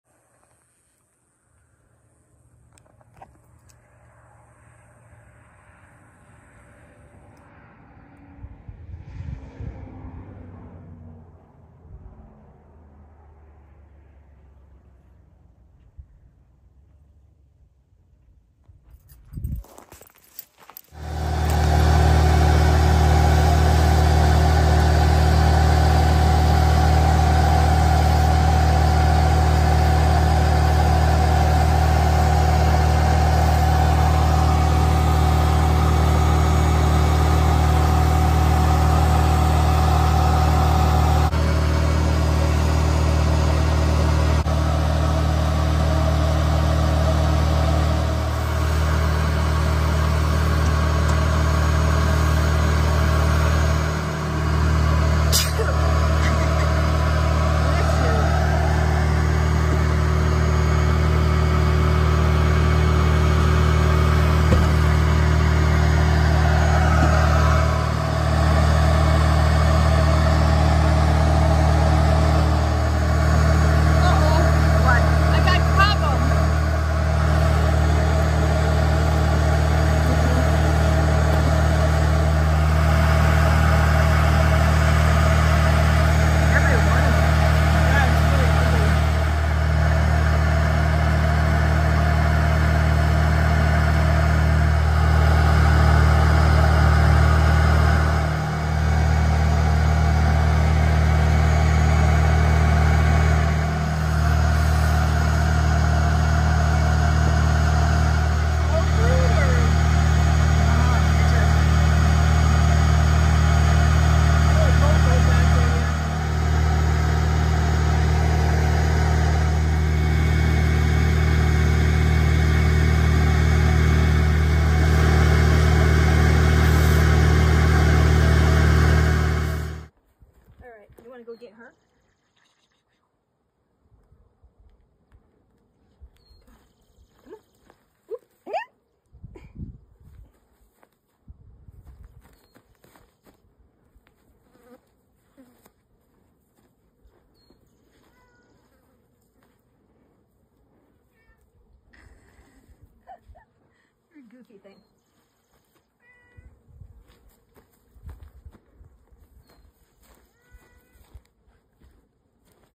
While the Grubsteak transforms into the Monarch, another beehive takes residence.
An old restaurant, run down building, called the grubsteak in Arivaca Arizona is getting a well needed transformation into the monarch. Soon to be Coffee Shop. ￼